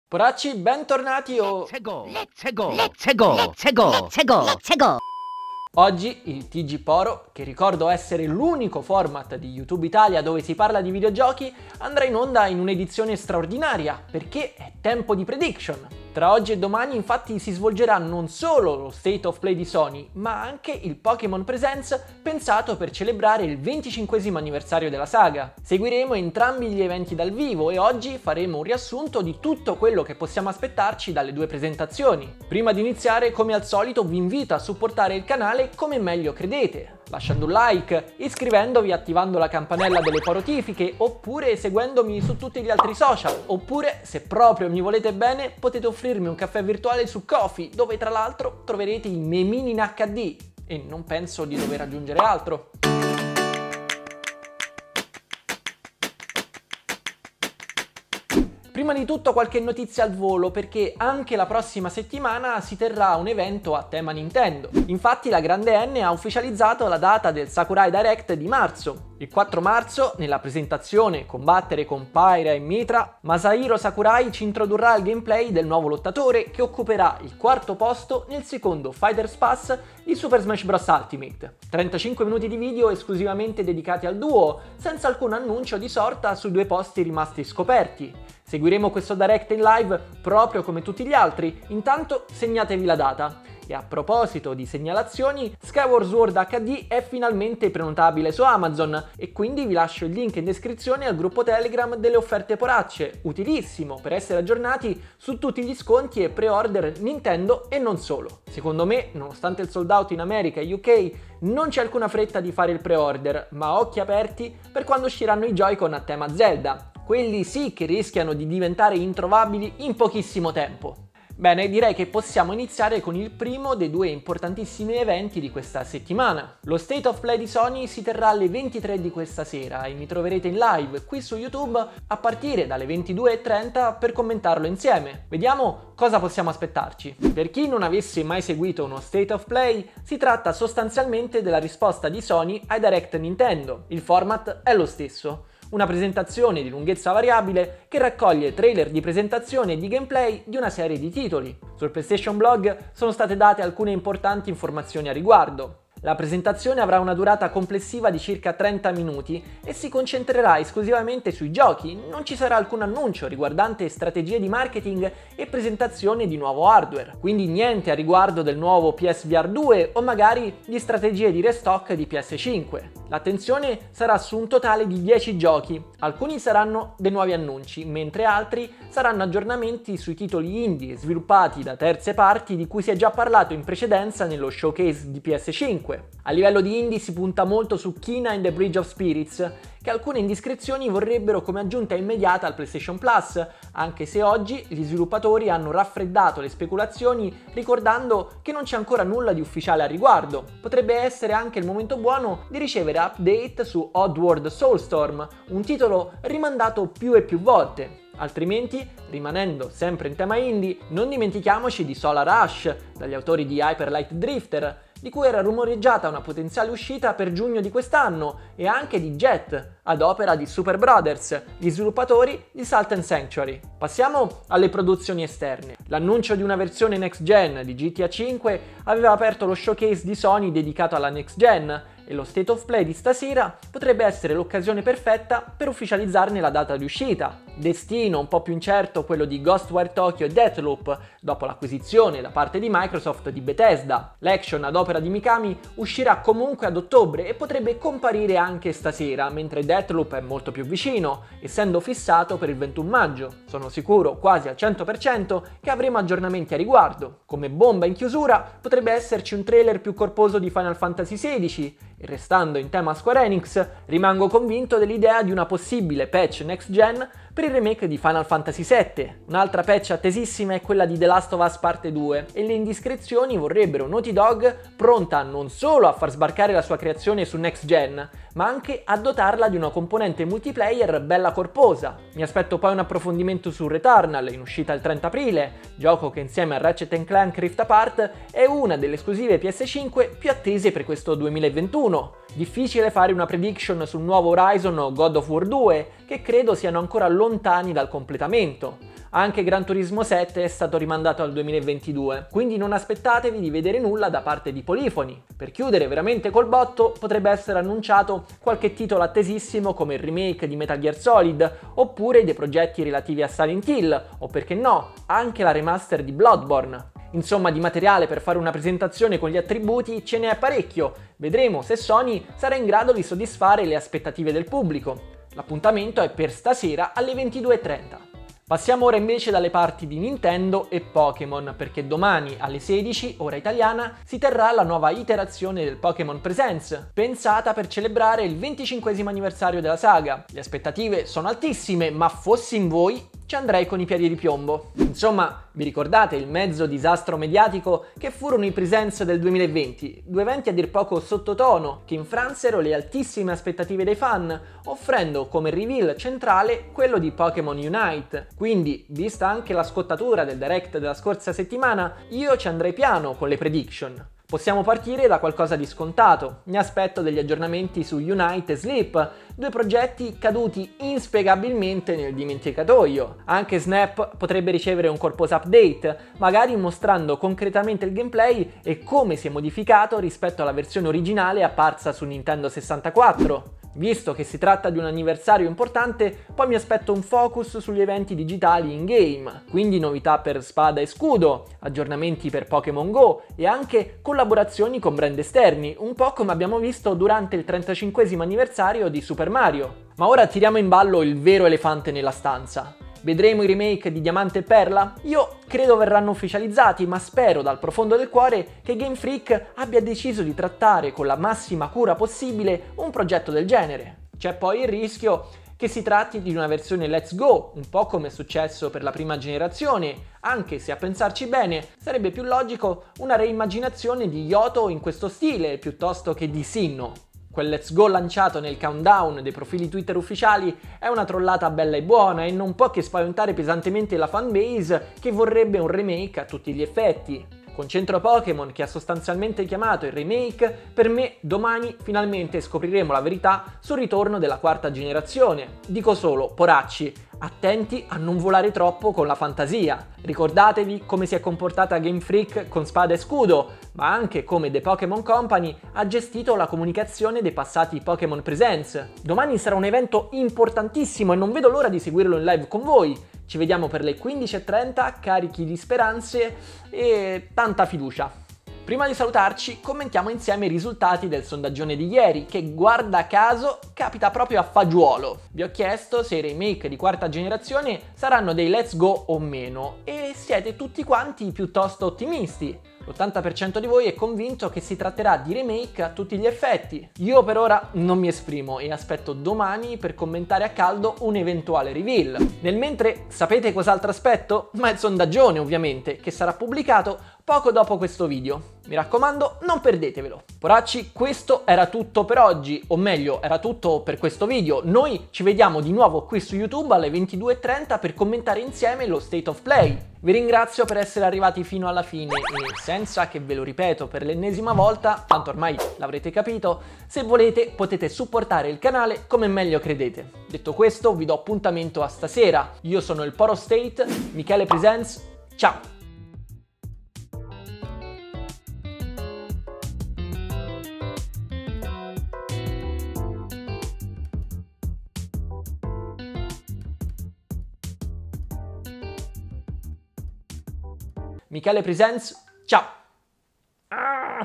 Cosa vedremo nello State of Play e nel Pokémon Presents? PREDICTION E SPERANZE | TG PORO
00:00 - Intro
01:04 - Combattere con Pyra e Mythra SSBU Direct
01:35 - Skyward Sword preorder
02:01 - State of Play
05:28 - Pokémon presents
07:49 - Sondaggione

Io sono il Poro State, Michele presents, ciao!